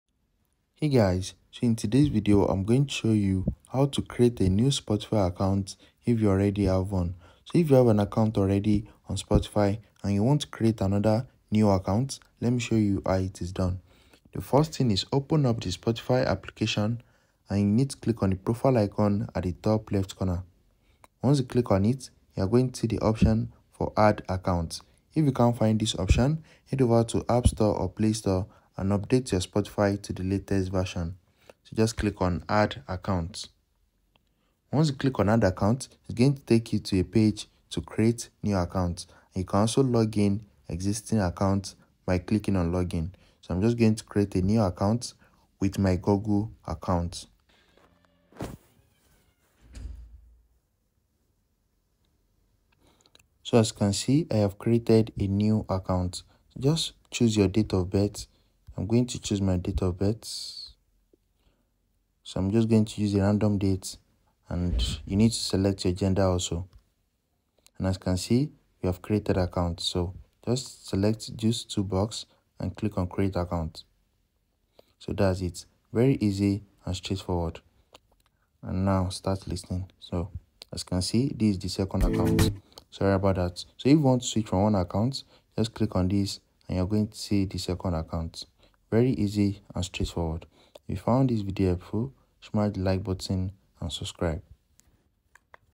How to Create New Spotify Account if You Already Have One
Want to create a new Spotify account even if you already have one? This tutorial will guide you through the simple steps to set up a second Spotify account, allowing you to have multiple profiles with different listening preferences. Learn how to create a new Spotify account and manage your multiple accounts effectively. Watch now and learn how to create a new Spotify account if you already have one.